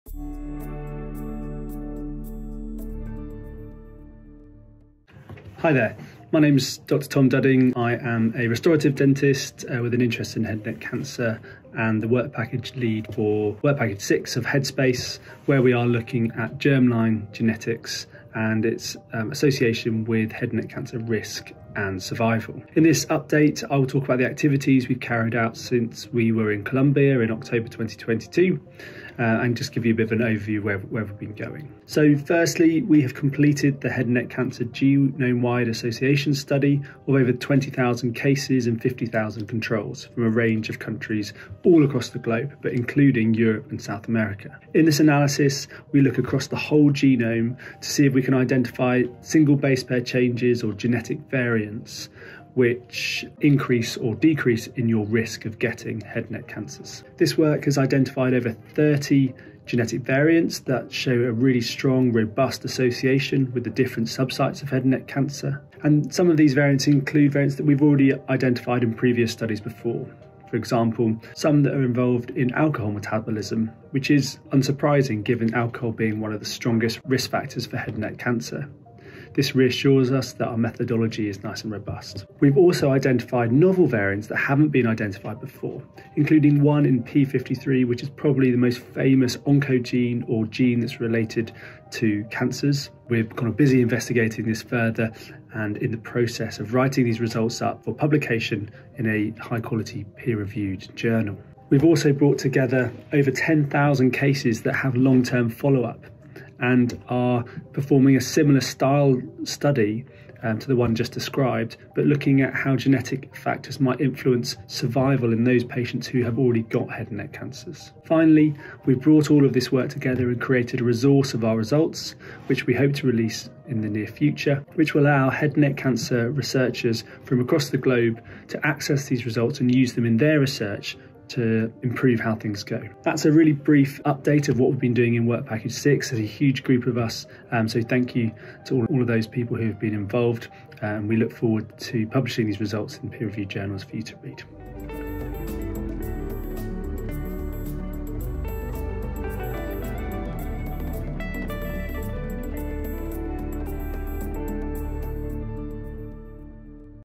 Work Package 6 Update
The leader of work package 6, Tom Dudding, give us a brief update on the group activities on the study